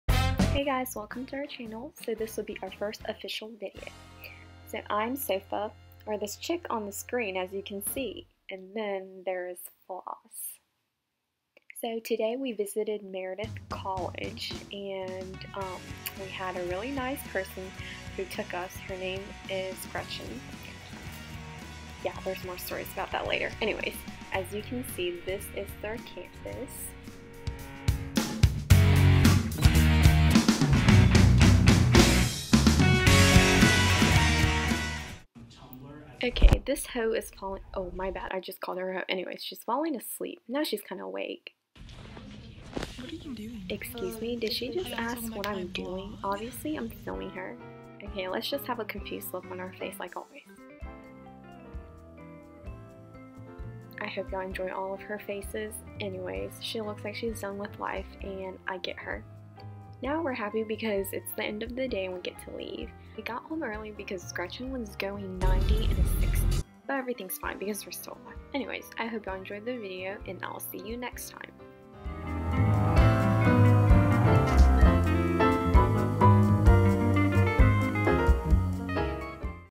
A Day at Meredith College
So we had fun at Meredith today and we wanted to share our story. This video may be boring, but we thought it was funny so yeah :)